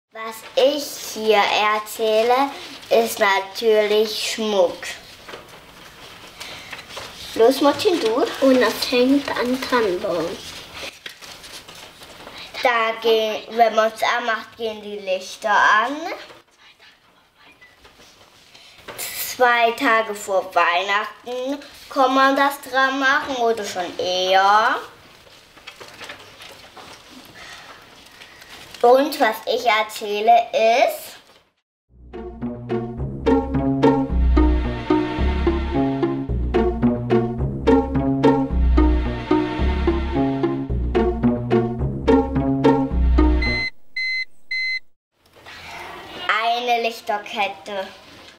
Dings da #12 - Rätsel [Februar 2020]
In Eilenburg hatten wir die Ehre mit der Kita Schwalbennest (DRK) eine Kleine Entdeckungsreise in die Medienwelt zu unternehmen. Was sind alles Medien und welche Sinne benutzen wir wofür waren aber nicht unsere einzigen Fragen. Auch kleine Filme wollten die Kinder produzieren. Und jetzt müssen die Erwachsenen mal schlau sein und sich Mühe geben! Also raten, rätseln, Spaß haben.

Alle Lösungen im Überblick

Bahnhofstraße 56
04720 Döbeln

Sächsischen Landesmedienanstalt